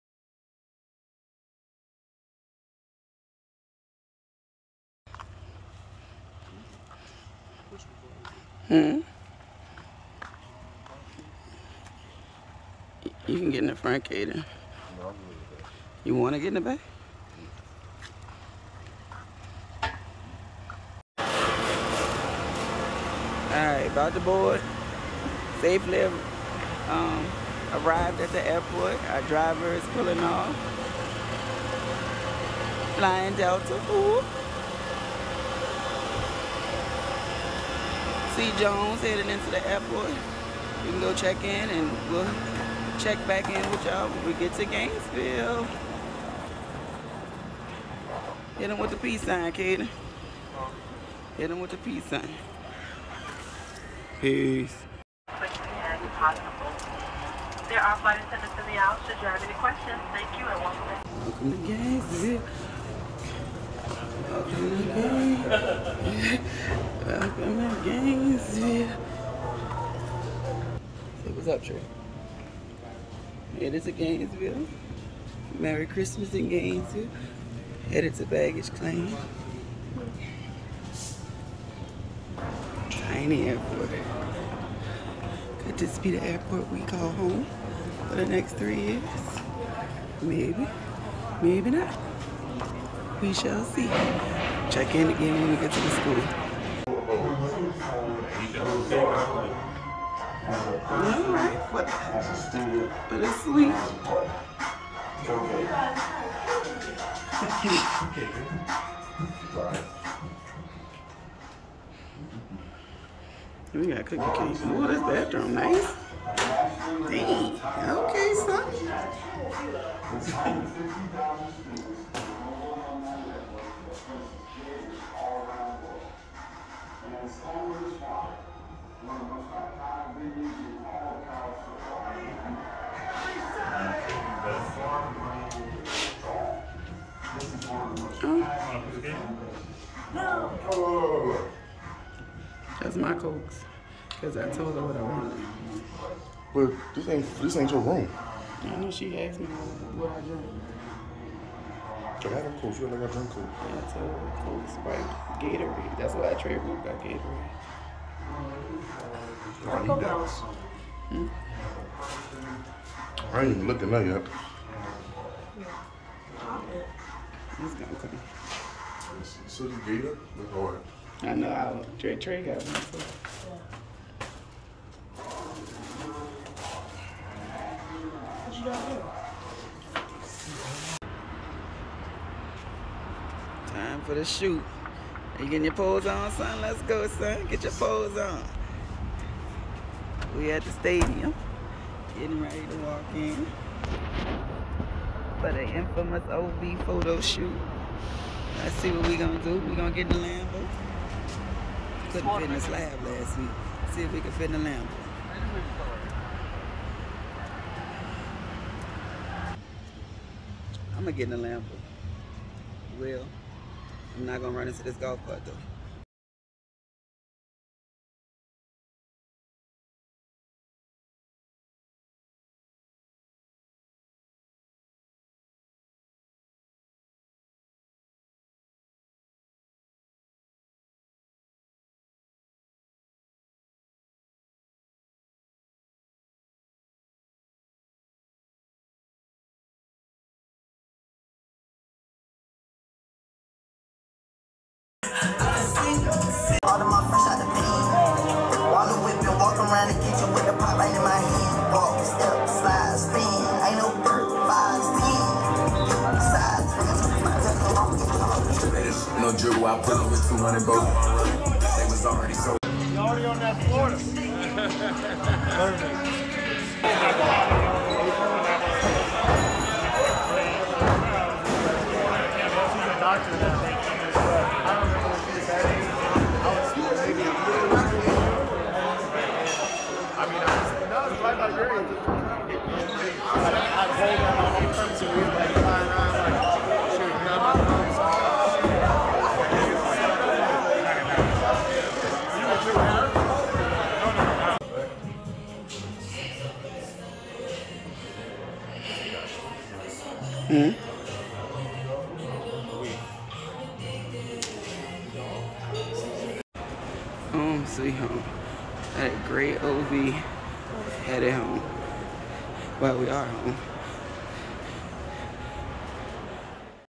Behind the Scenes: Official Visit Weekend Visit with Caden Jones
Inside the Gators got a behind-the-scenes look as offensive tackle Caden Jones took his official visit to The Swamp. Out of Louisiana, Jones is a top-30 OT according to On3 where he is listed as a three-star in their consensus ranking.